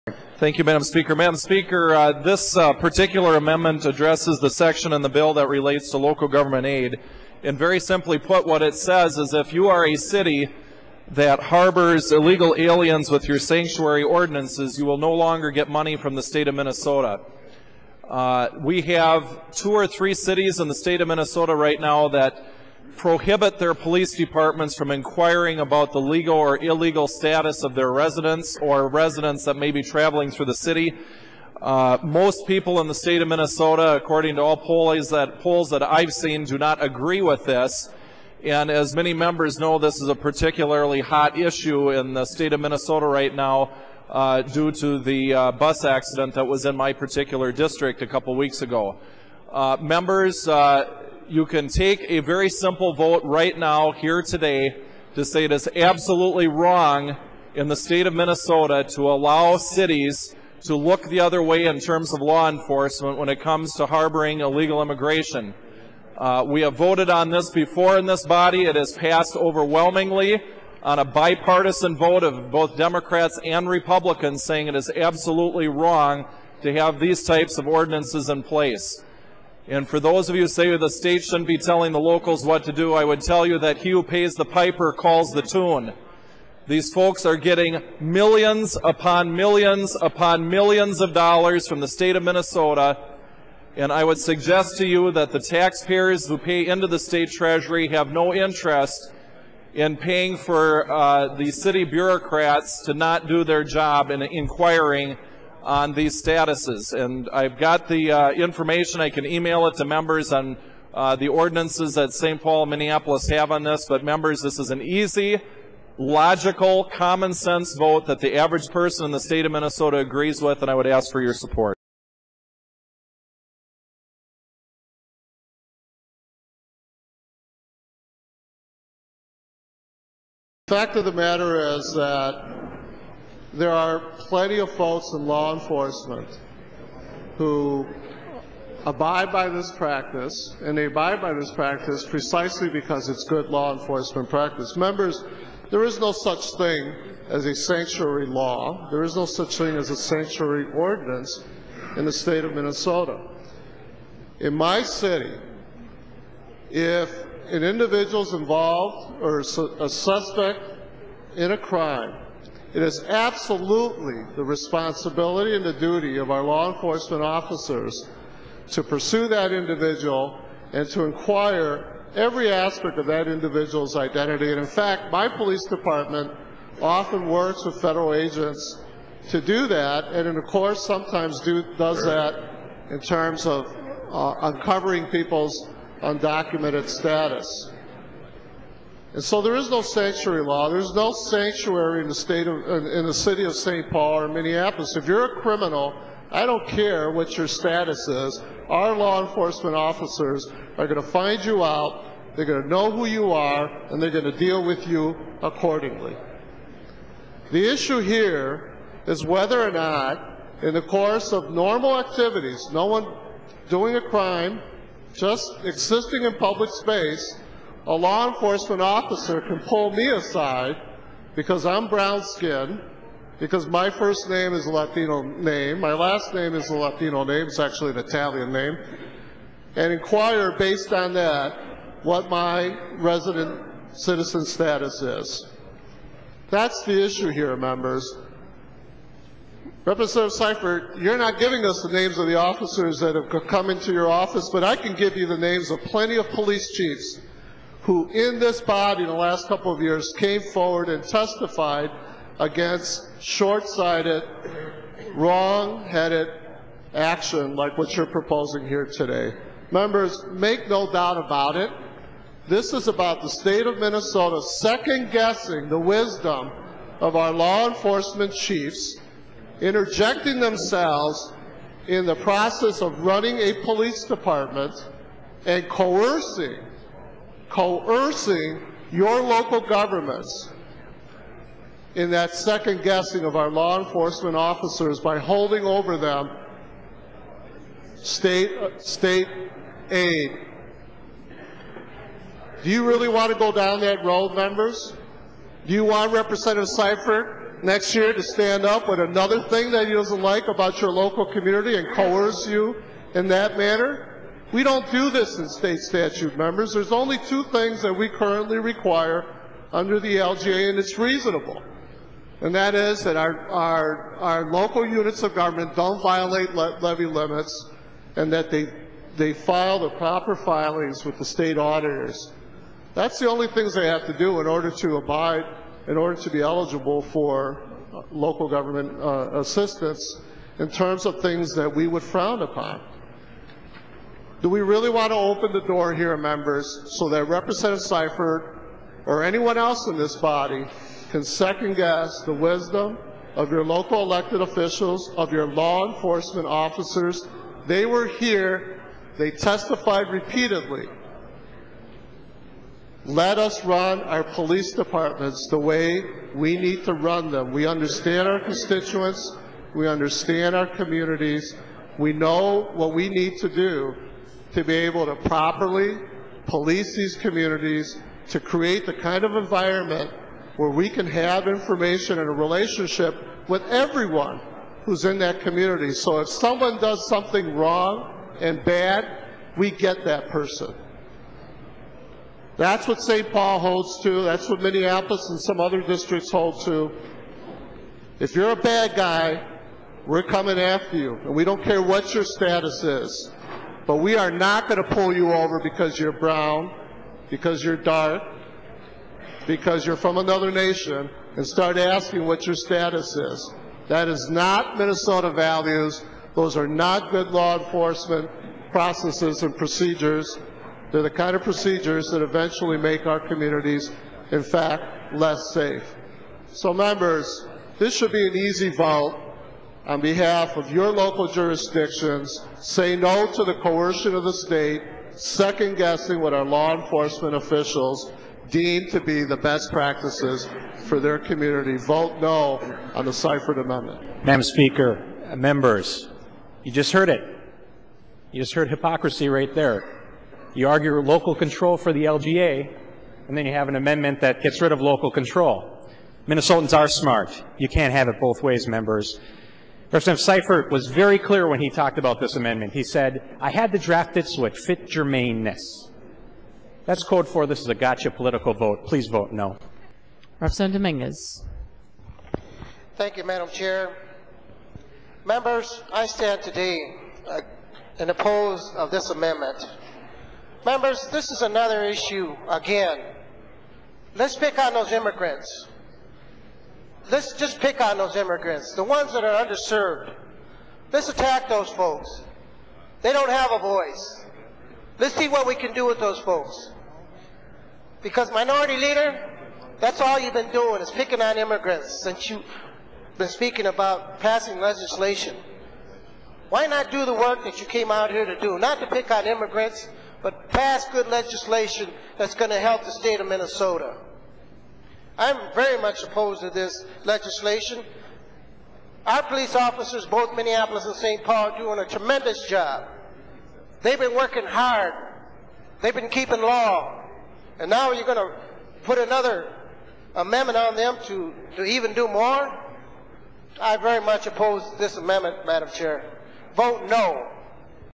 Seifert Demagoguery
Marty Seifert, in the Minnesota House of Representatives, chose to use what he termed a "hot" issue in order to attack the police enforcement practices of Minneapolis and St. Paul. His attempts at demagoguery were met with several smackdowns from opponents to his tactics.